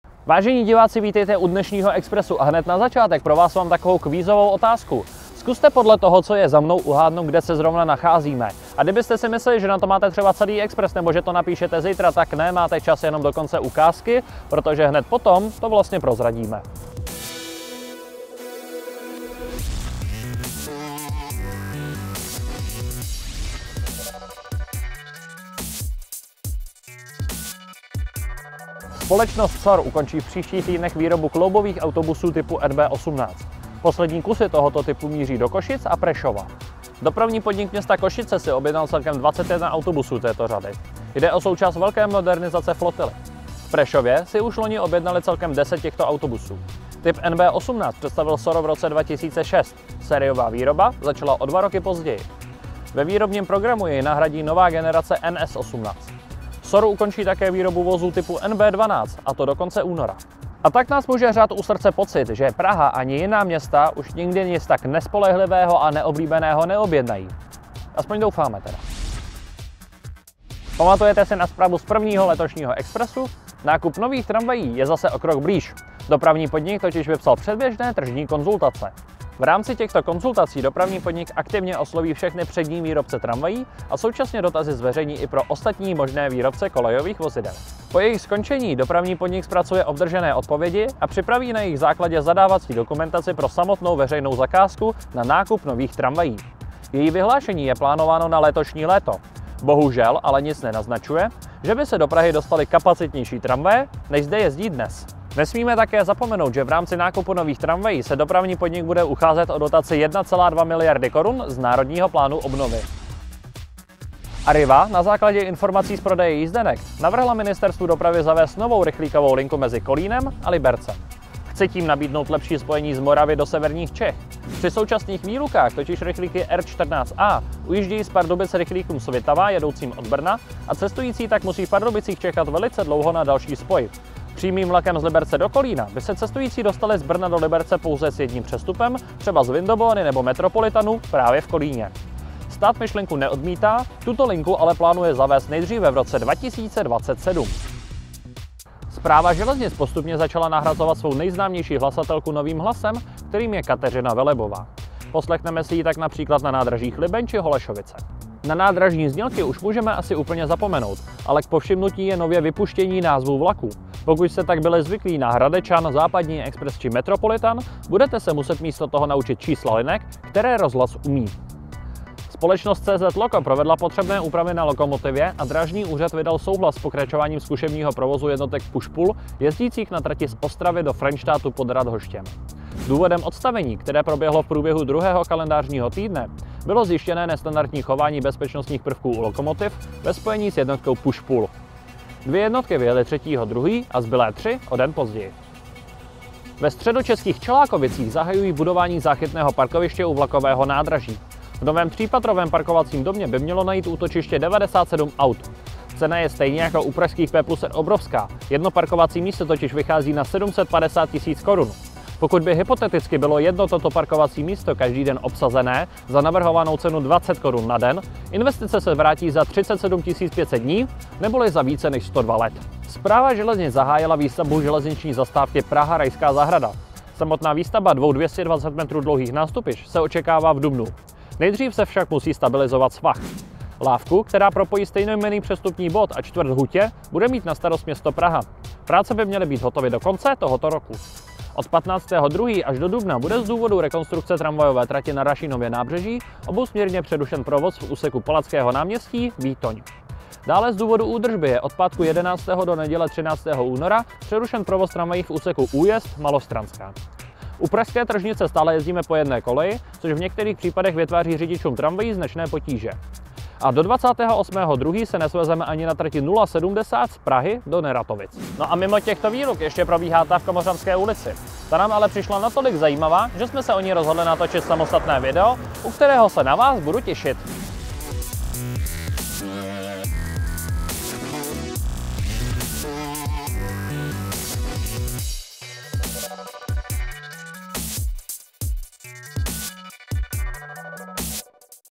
METROBUS EXPRES 2022#3: Staré končí a nové začíná
Z východočeských Libchav vyjíždí poslední kusy SORů typové řady NB, které v minulém desetiletí zaplavily nejen Prahu. Co bude následovat? Nové tramvaje?

V Expresu #3 uvidíte:
SOR končí s řadou NB (zdopravy.cz)
Tržní konzultace k tramvajím (zdopravy.cz)
Nový rychlík z Liberce (zdopravy.cz)
Nový hlas na železnici
Push-pully (zdopravy.cz)
P+R Čelákovice (zdopravy.cz)
Zastávka Rajská Zahrada (TW Správa železnice)

Časové značky:
0:00 Úvod
0:21 Staré končí
1:09 Nové začíná
1:53 Nový rychlík z Liberce
2:27 Nový hlas na železnici
2:51 Pushpully se vrací na scénu
3:16 P+R v Čelákovicích
3:42 Železniční zastávka Praha-Rajská Zahrada
4:00 Výluky
4:28 Závěr